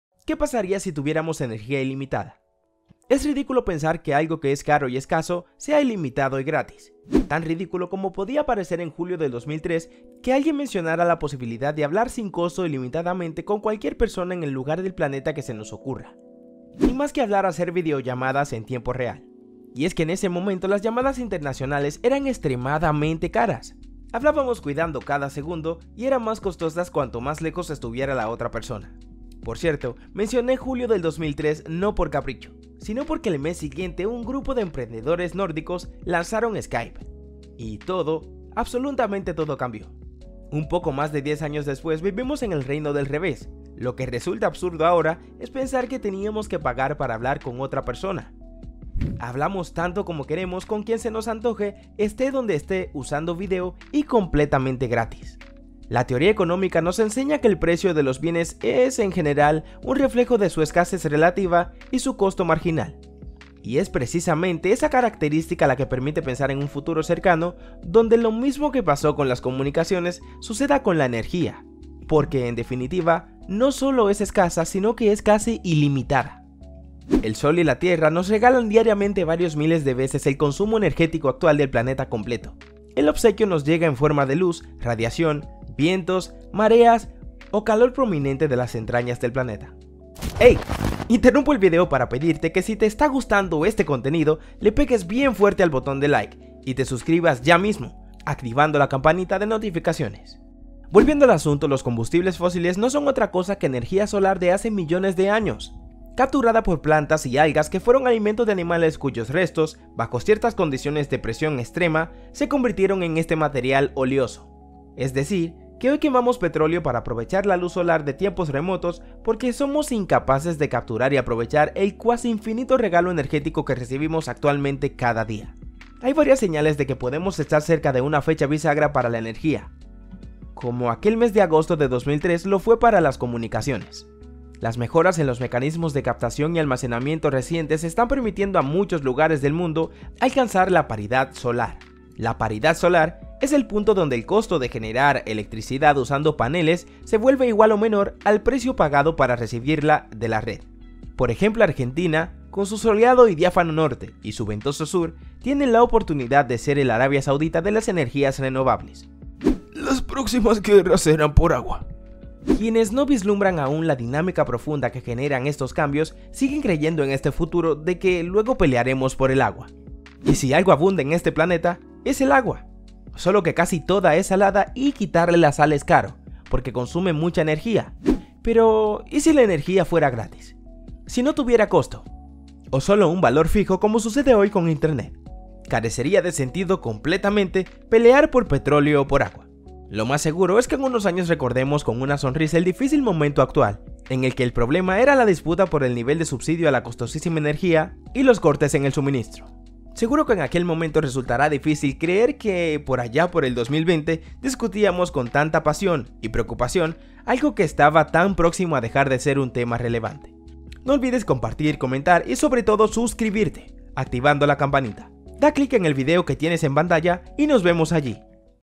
Energia Infinita ¿ES POSIBLE? ⚡🔌
¿Free energy? ¿Es posible conseguirla con motores, imanes, o con magie de hogwarts? ¿Que tantas cosas podemos hacer con esto?

Fuente de información:

Algunos vídeos de Pexels y otros creadores de YT.
Y algunos recortes del vídeo de la web de la fuente.